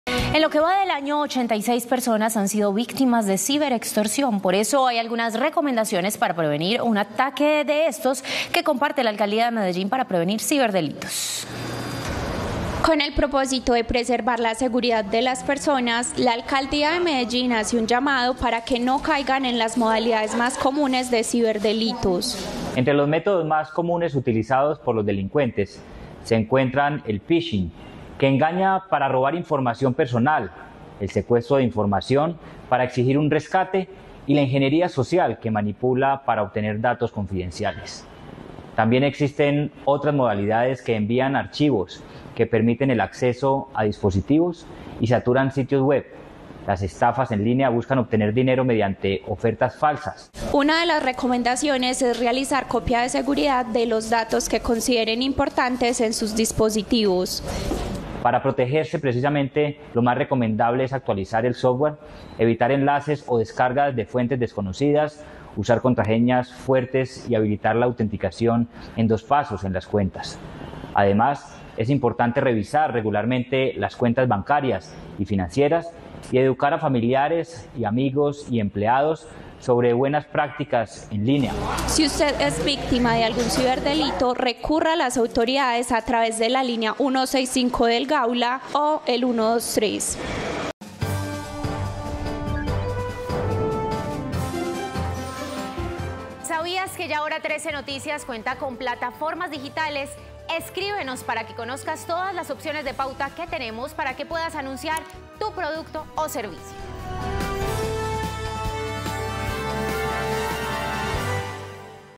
¡Tenga cuidado! 86 personas han denunciado ser víctimas de ciberextorsión este 2024 en Medellín
La Alcaldía de Medellín entrega recomendaciones para prevenir ciberdelitos.